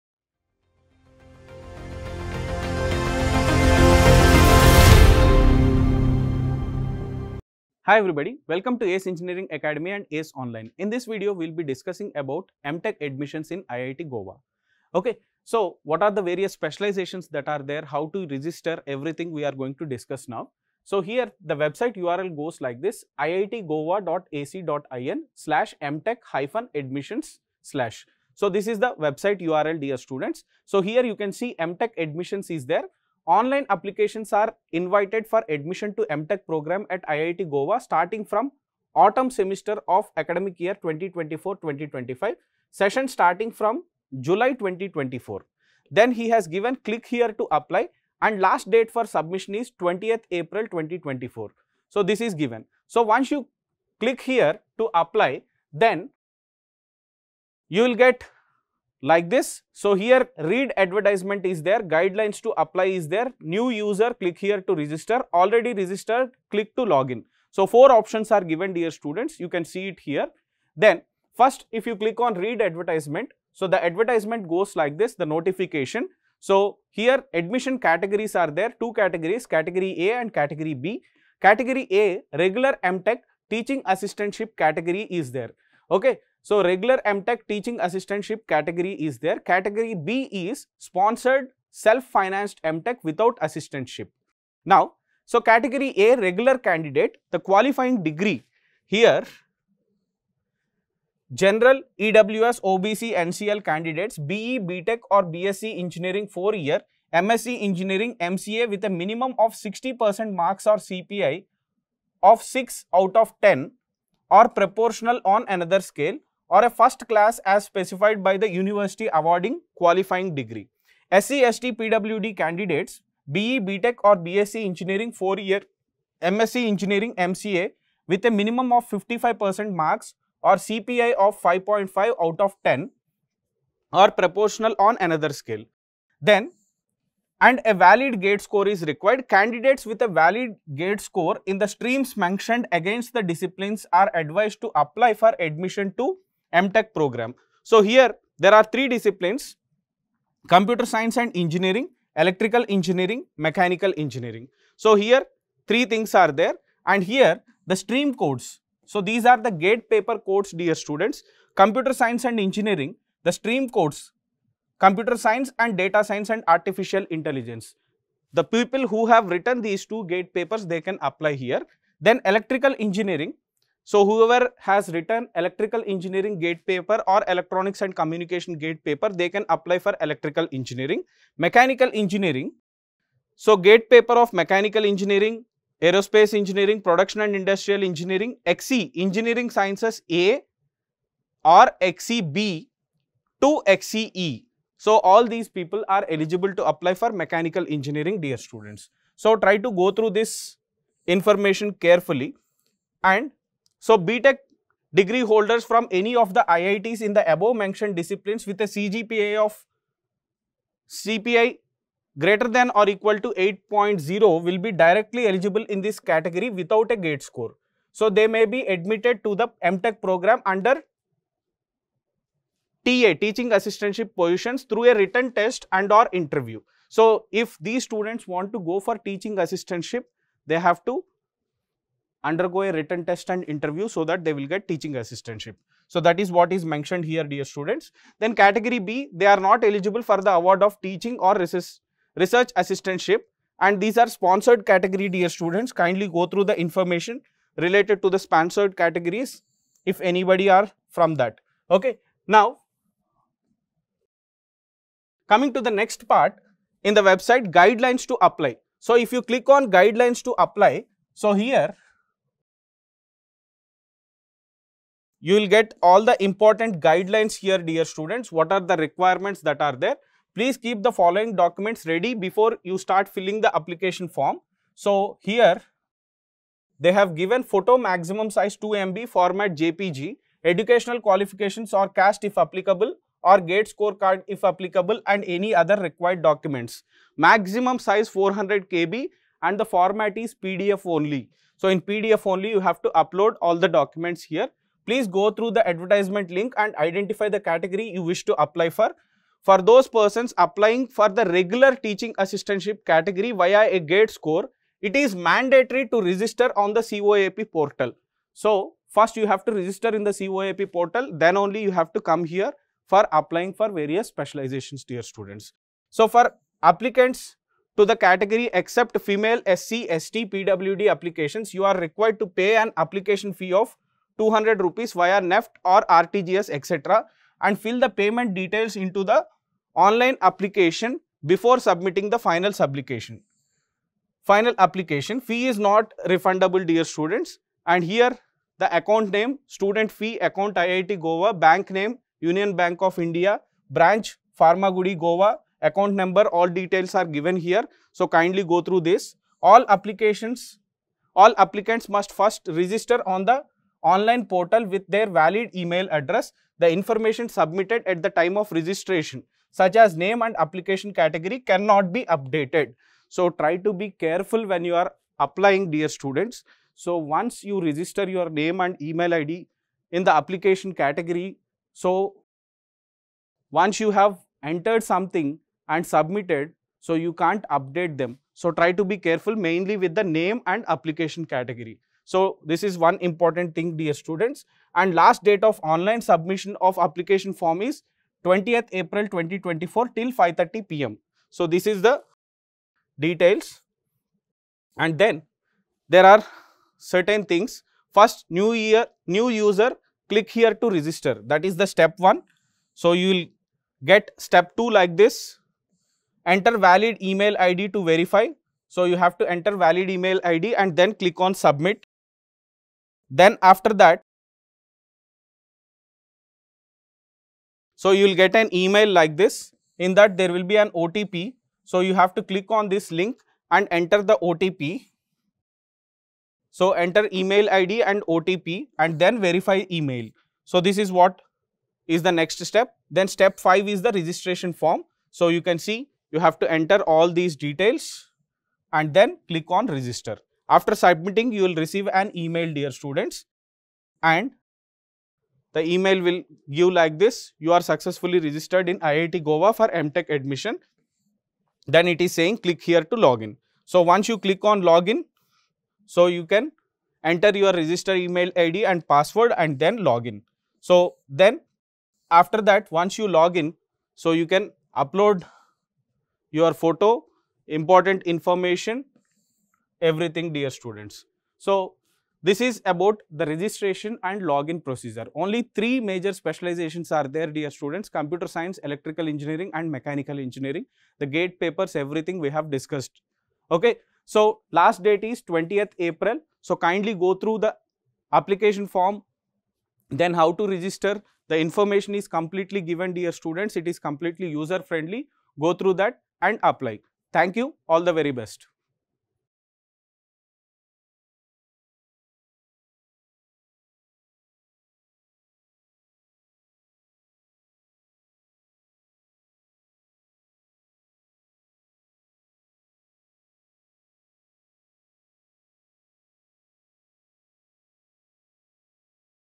IIT Goa M.Tech Admissions 2024 | Post GATE Counseling Sessions | Selection Process & Fee Structure
Get ready for IIT Goa M.Tech Admissions 2024. Join ACE Online for exclusive Post GATE Counseling Sessions where we delve into the intricacies of admissions at IIT Goa. Gain insights, tips, and expert guidance to navigate through the admission process smoothly. Our experienced counselors will provide comprehensive information about the M.Tech programs, eligibility criteria, important dates, and much more. Whether you're a GATE aspirant aiming for IIT Goa or simply seeking valuable information about post-GATE counseling, this session is for you. Don't miss out on this opportunity to kickstart your journey towards a successful M.Tech career at one of India's premier institutes.